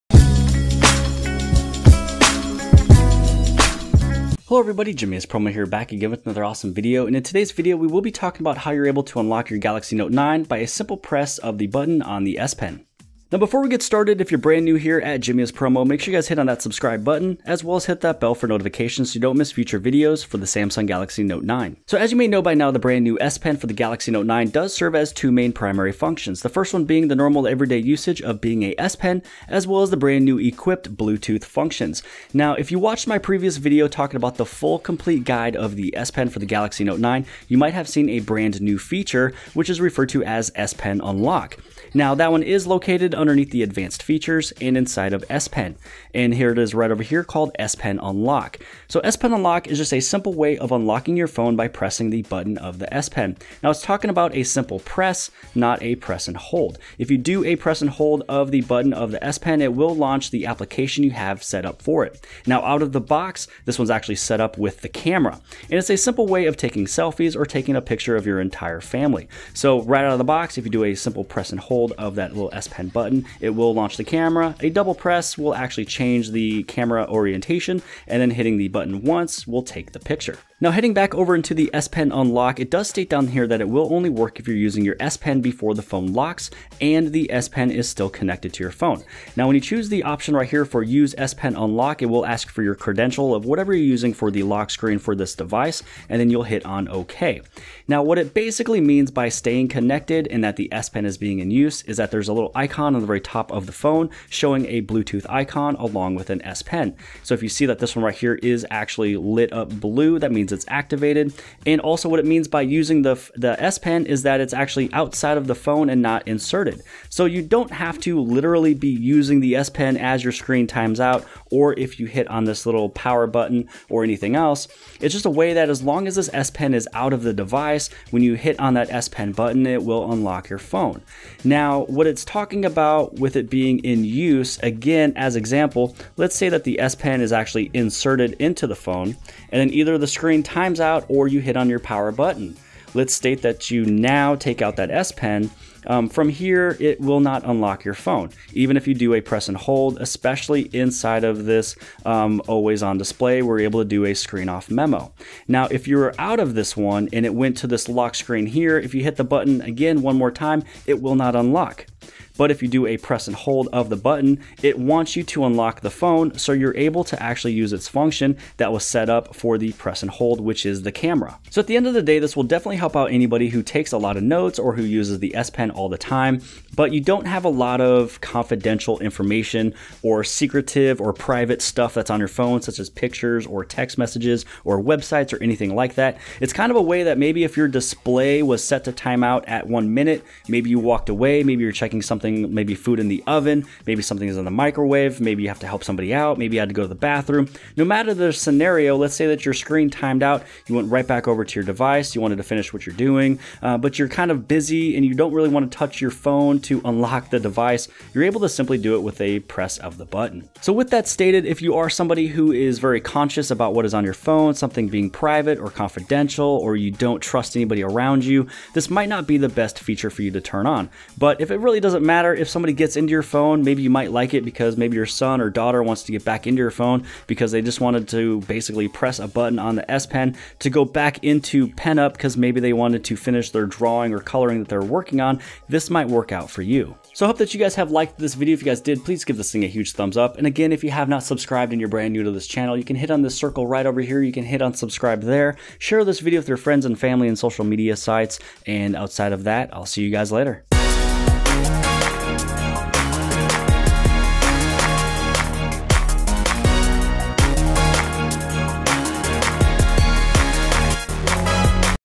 UNLOCK Your GALAXY NOTE 9 with The S Pen Button
Welcome to the home of the best How-to guides for your Samsung Galaxy needs. In today's video we will be going over a new function of the Galaxy Note 9. With the S Pen, you are able to unlock your phone via the New Bluetooth S-Pen.